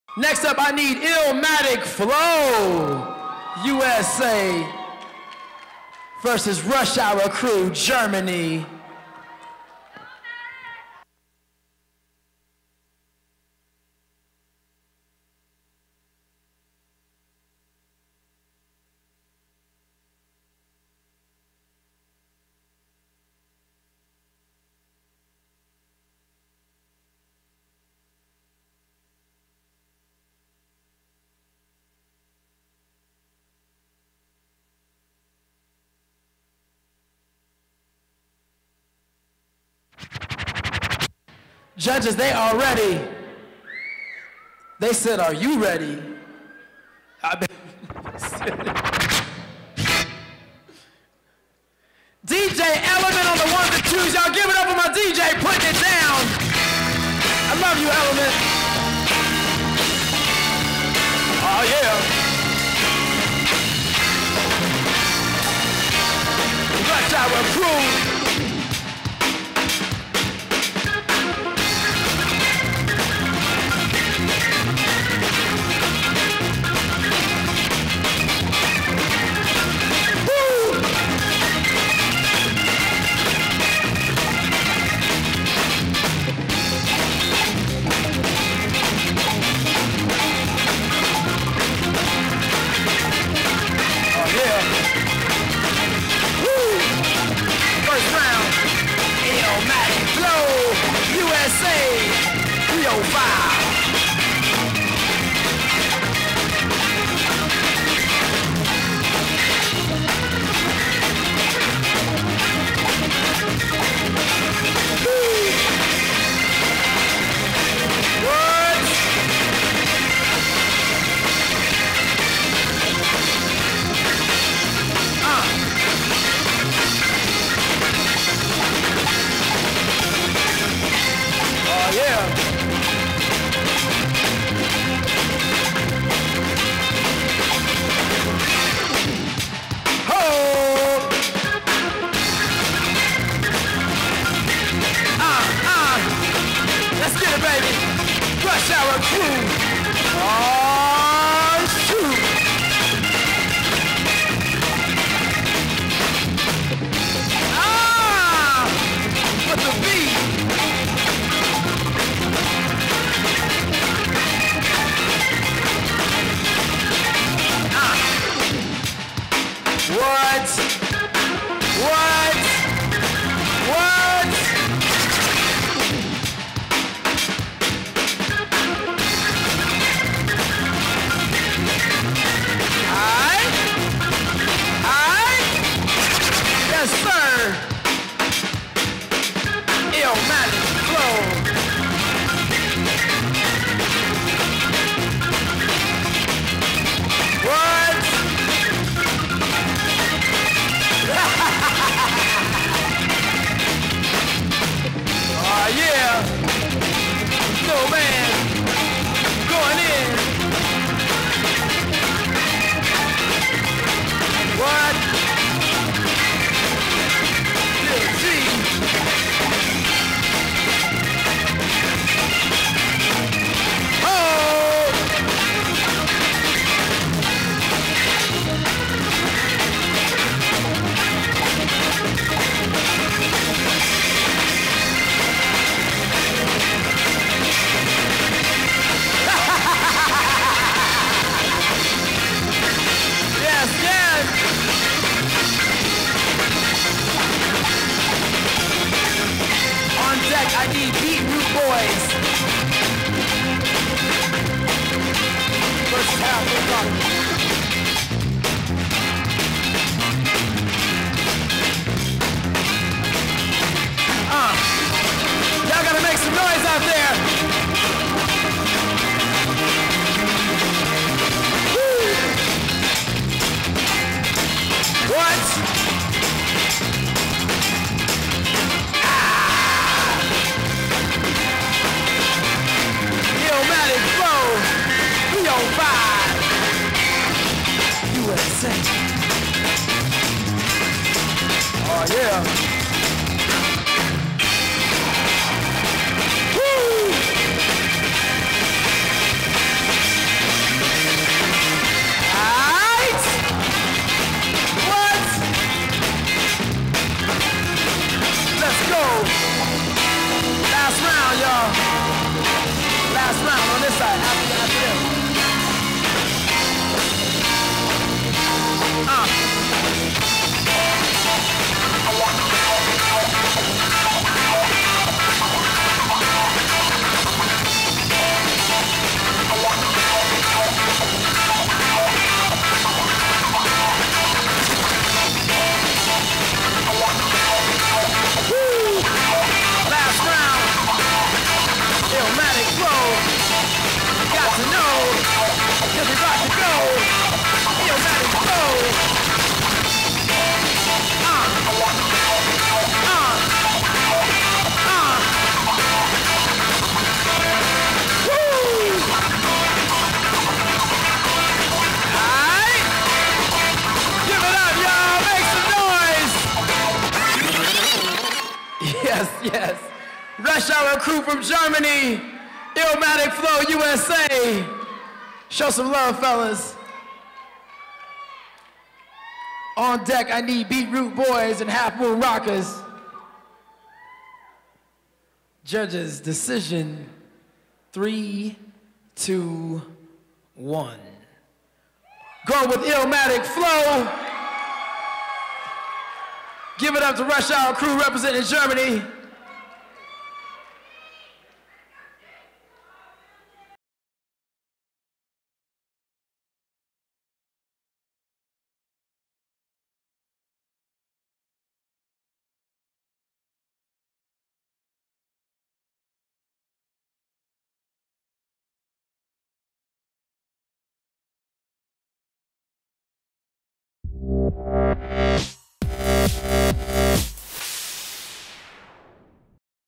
Illmatik Phlow - USA vs Rush Hour Crew - Germany | @ HHI's World Bboy/Bgirl Battles!!
Round 1 (Top 16) | Illmatik Phlow - USA vs Rush Hour Crew - Germany @ Hip Hop International's 2012 World Bboy/Bgirl Battles.  Held in conjunction with Hip Hop International's 2012 USA @ World Hip Hop Dance Championships.  August 4th in Las Vegas!!
Battle Coordinator: Zulu Gremlin Judges: EasyRoc, IceMan & Machine